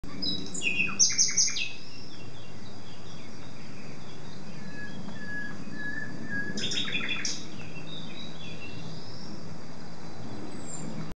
Birds song / Chant d'oiseau
Birds singing in the trees / Des oiseaux chantant dans les arbres.